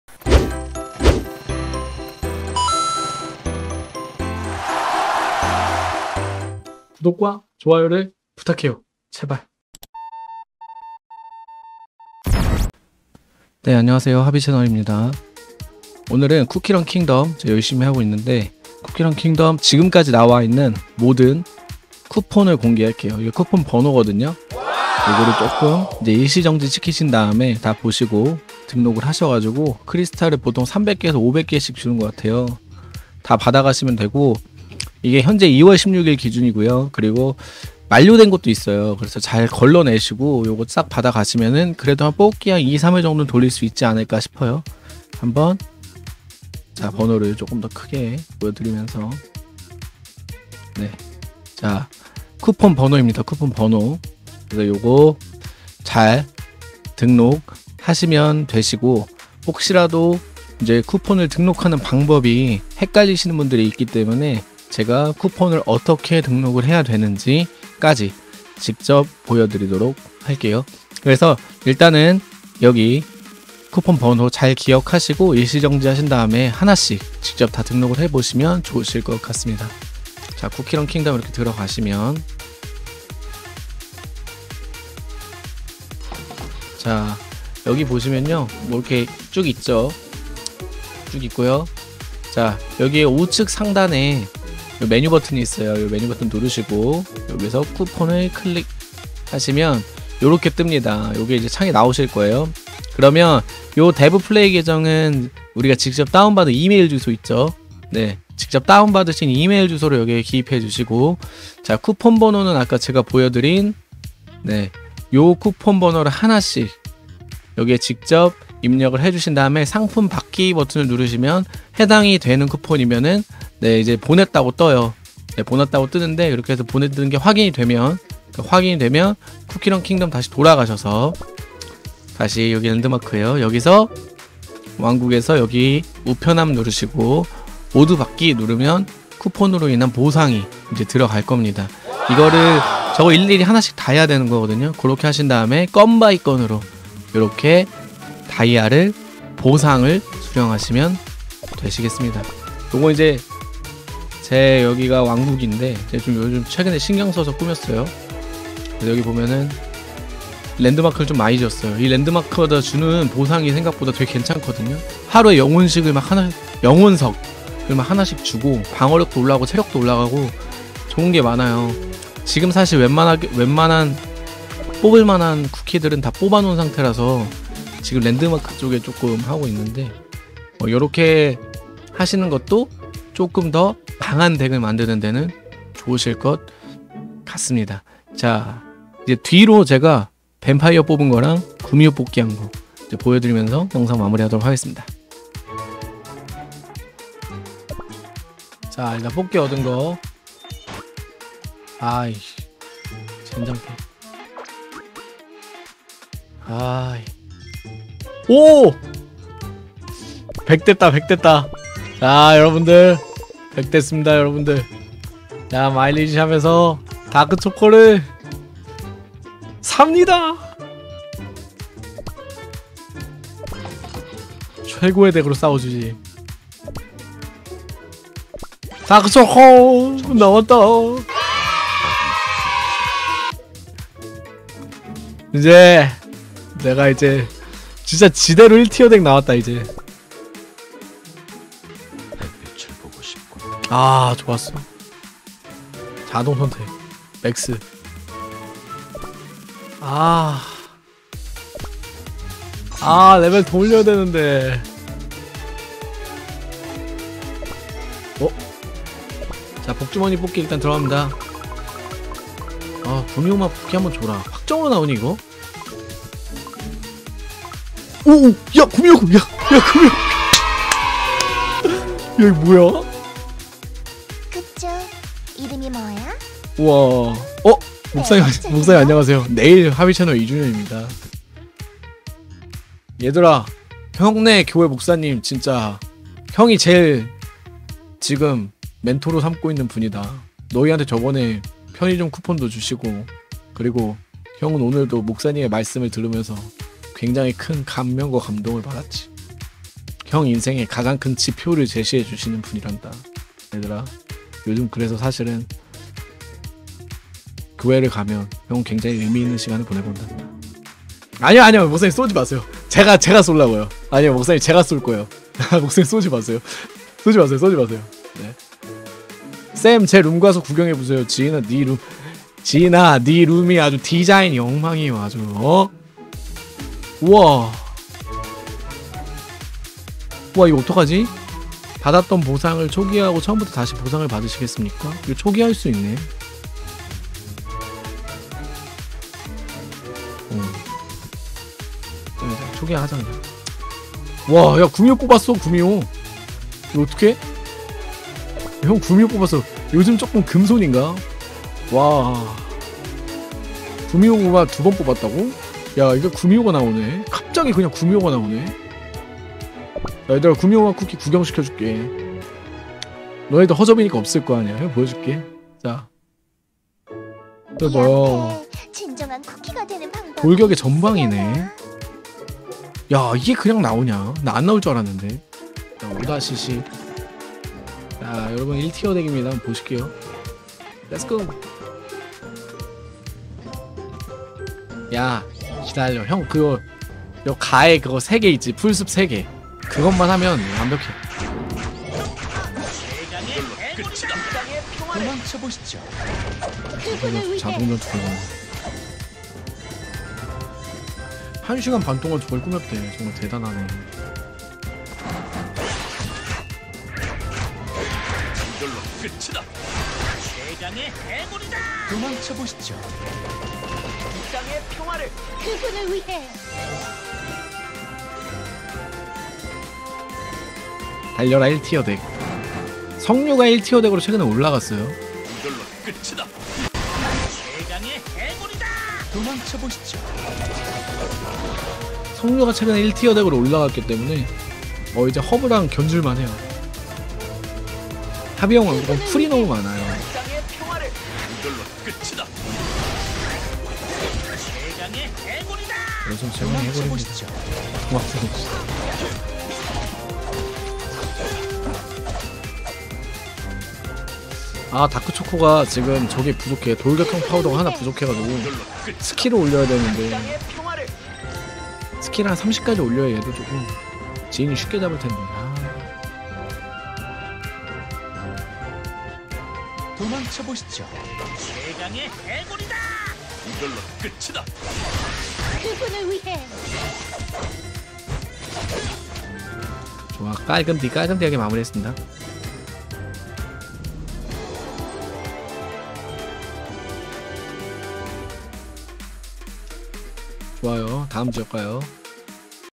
쿠키런 킹덤 ★최신★ 다이아 쿠폰공개! 쿠폰번호부터 등록방법까지 쉽게 알려드립니다!
★ 하비형의 일상은 "하비이즈프리"에서 할때도 있습니다. 구독띠

쿠키런 킹덤 최신판 다이아 쿠폰번호 공개합니다!
등록방법까지 자세히 알려드리니 영상 꼭 시청하세요~